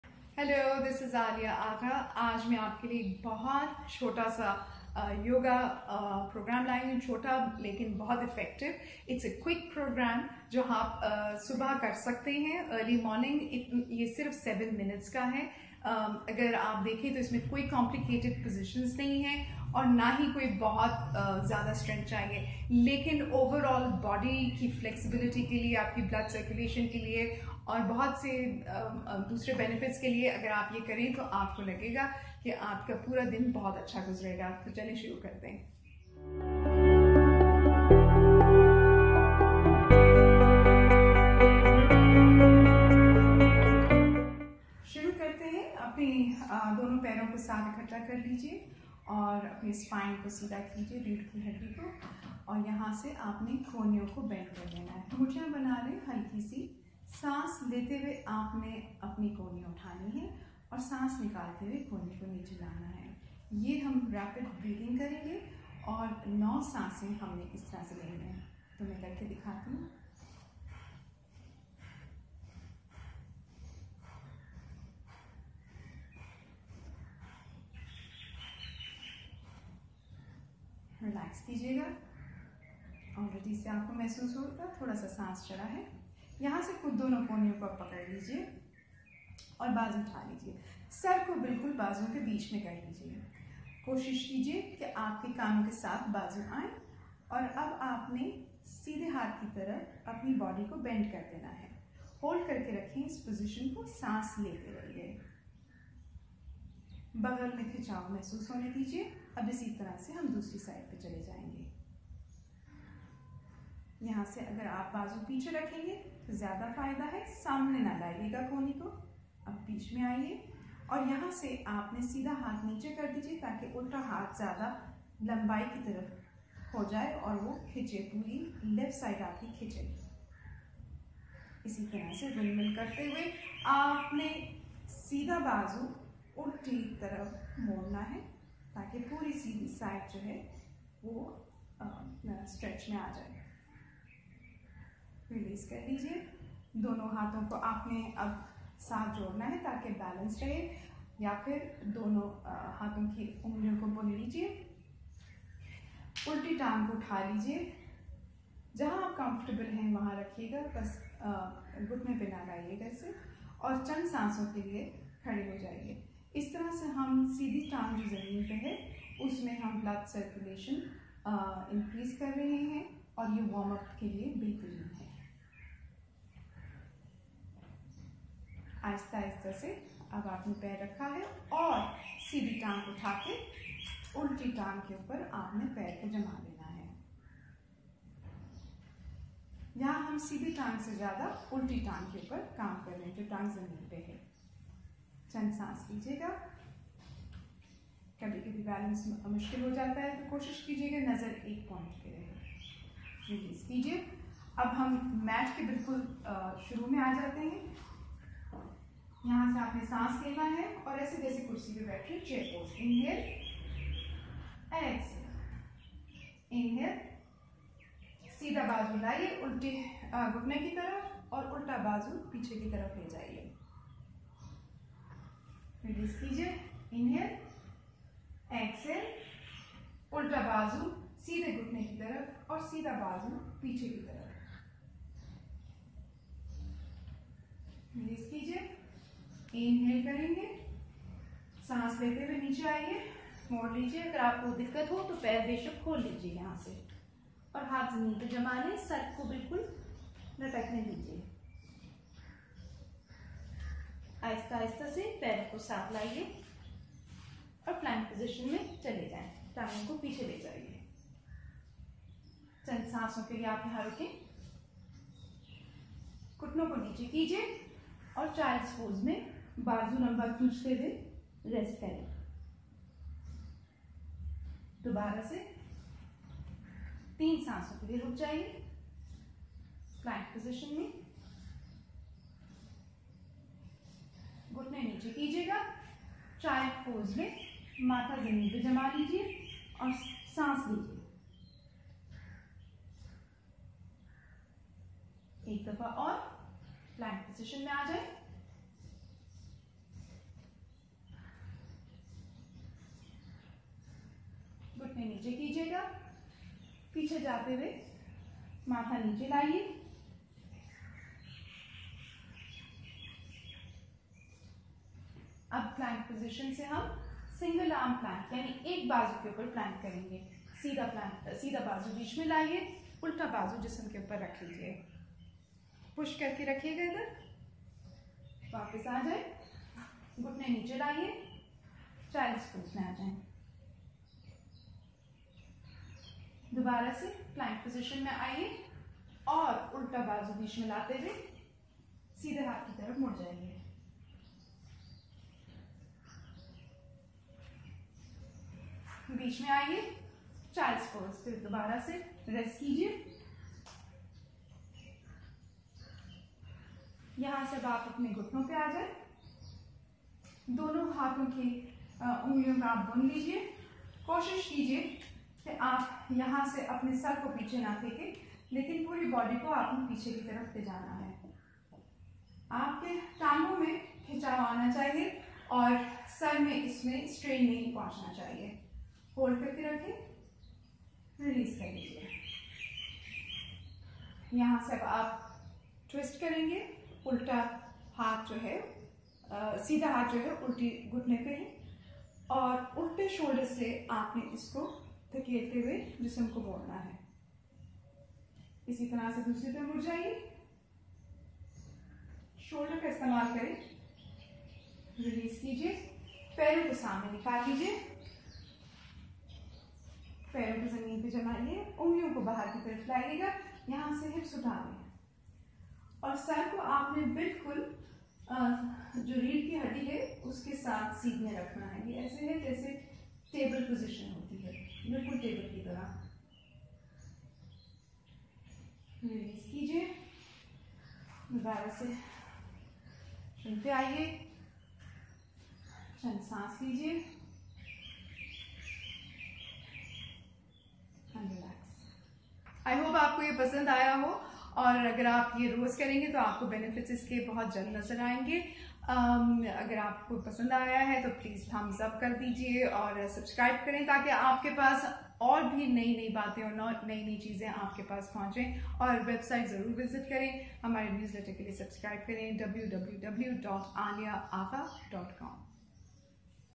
7 Minute Beginners Yoga
This is a simple, easy and healthy routine to start your day with yoga. You will see wonderful difference in your mental clarity and body rhythms.  Leave a comment for us to know whats on your mind.

Instragram: Aliya Agha